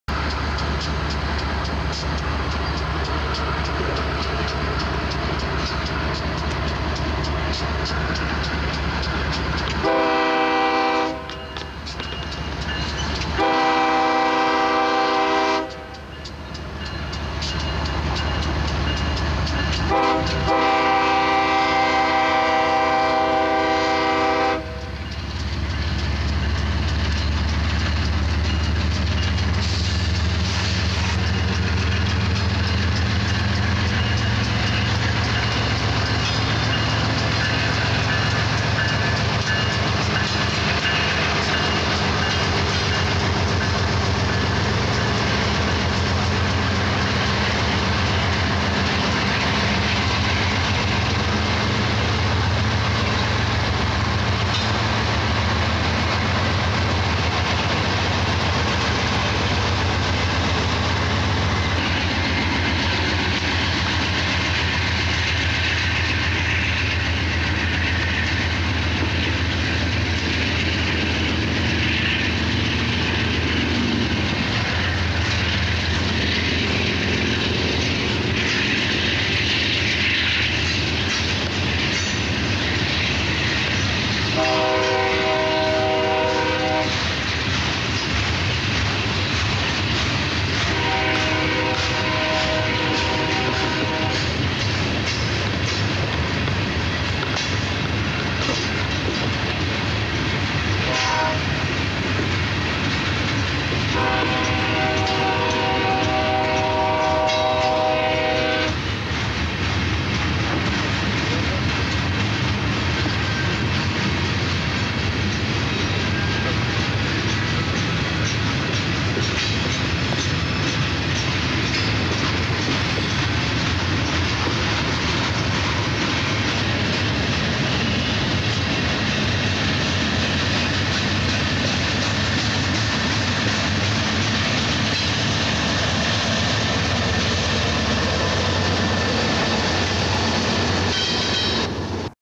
BNSF 7544 With Coil Train Departs Caliente
Set To High Quality

Freshly painted BNSF 7544 departs Caliente with a heavy coil train requireing 5 units to get it up the hill.